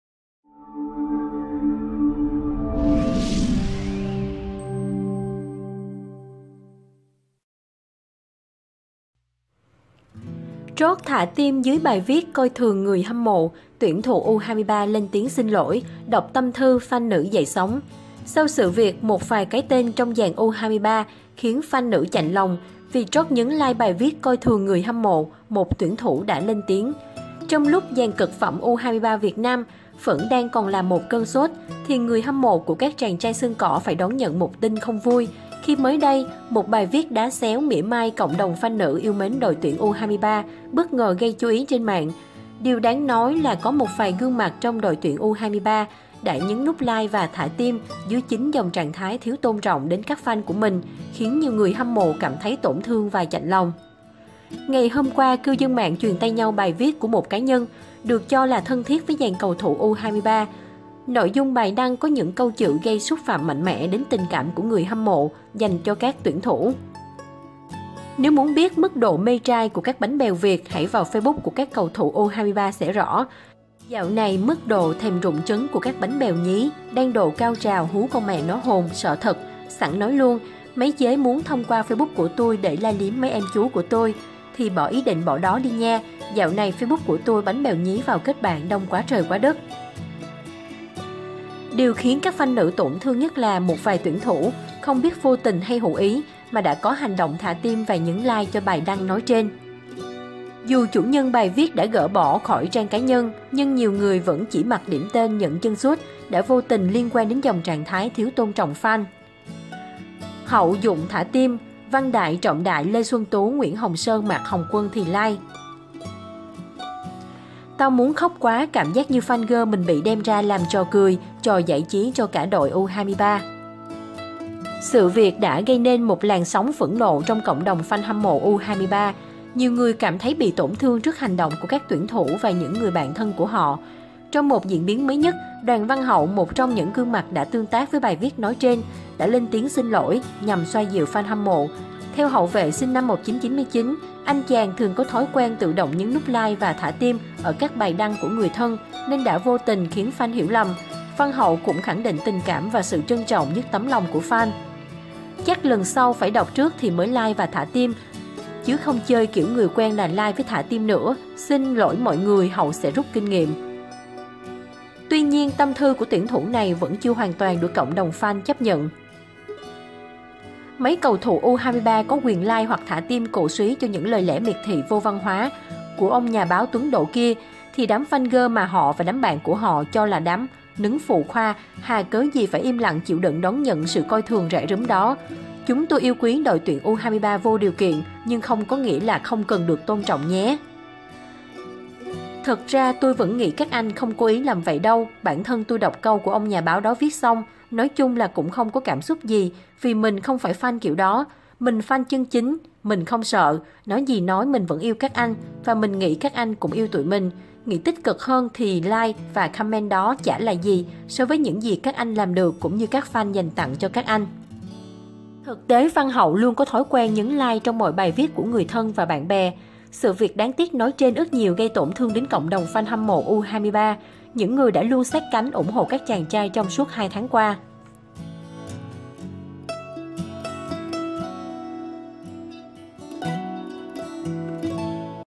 Cầu thủ U23 Việt Nam xin lỗi người hâm mộ vì trót thả tim bài viết coi thường Fan hâm mộ | Tin Nhanh
Cầu thủ U23 Việt Nam xin lỗi người hâm mộ vì trót thả tim bài viết coi thường Fan hâm mộ.